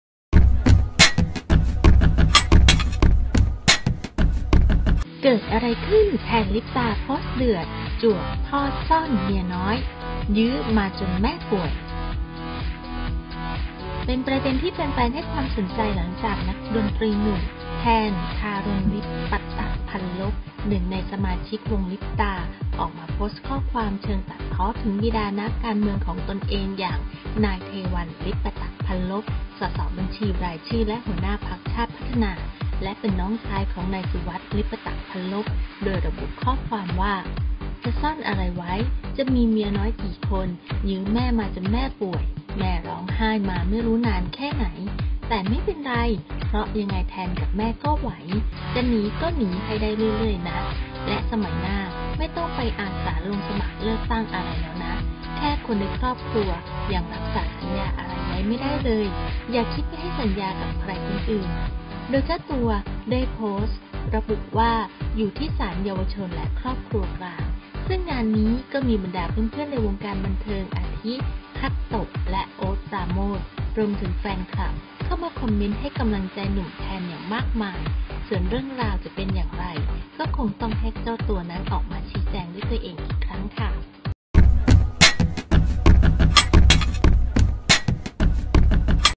เกิดอะไรขึ้น! “แทน ลิปตา” โพสต์เดือด จวกพ่อซ่อนเมียน้อย ยื้อมาจนแม่ป่วย
แทน ลิปตา